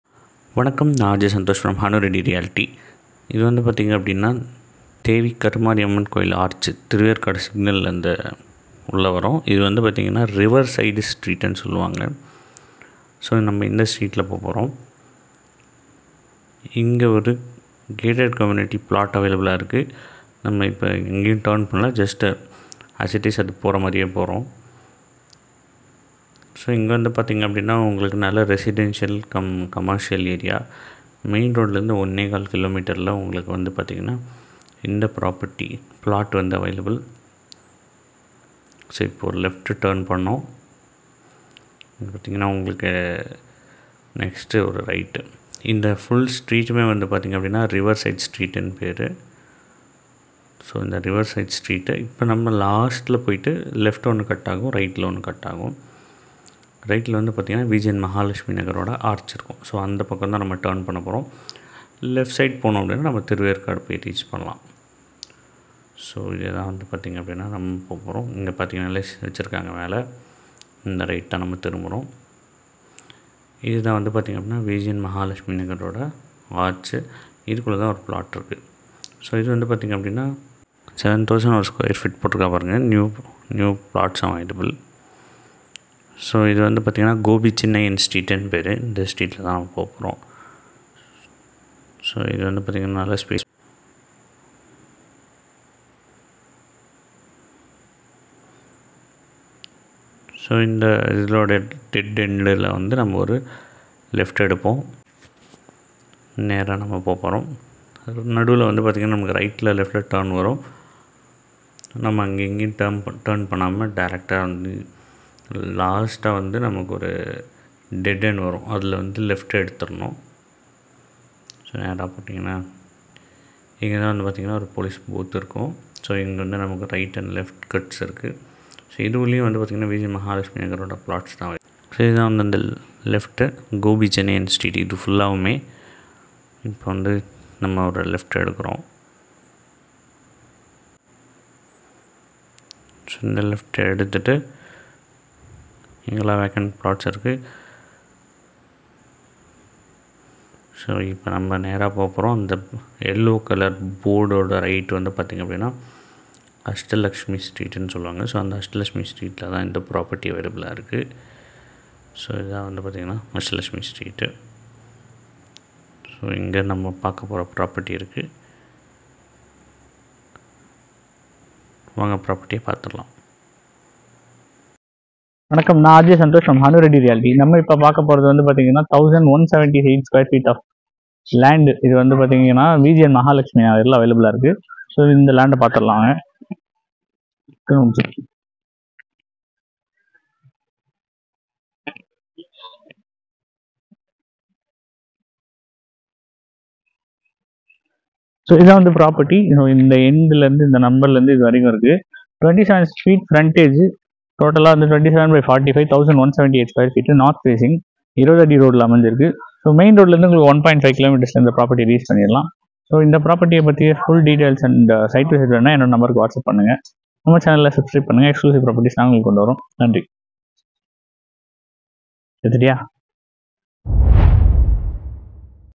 Thiruverkadu plots budget plots in chennai VGN Mahalakshmi Nagar, Chennai 1178 Sqft
CMDA Approved Plots @ Thiruverkadu, VGN Mahalakshmi Nagar

1.2 Kms from NH , Bangalore - Chennai Highway
Plot Area - 1178 Sqft
20ft Road
27 ft frontage
North Facing
Near Park
VGN Mahalakshmi nagar